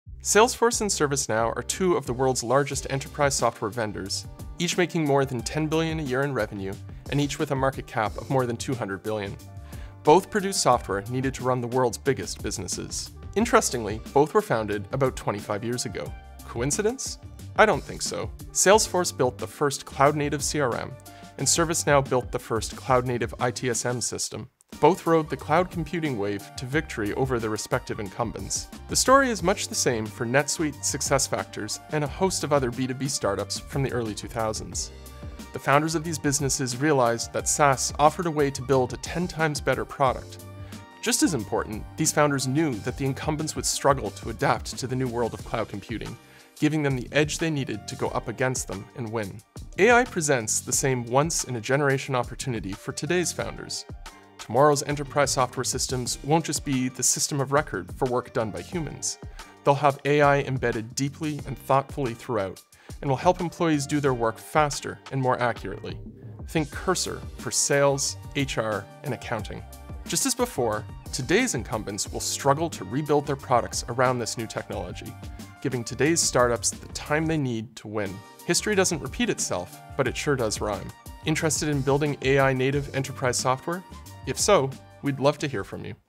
AI Native Enterprise Software
Salesforce and ServiceNow won by going all-in on the cloud before the incumbents could react. AI is the next wave—and it's just as big. The next generation of enterprise software won't just record work, it'll help do it. If you're building AI-native tools for the enterprise, let's talk.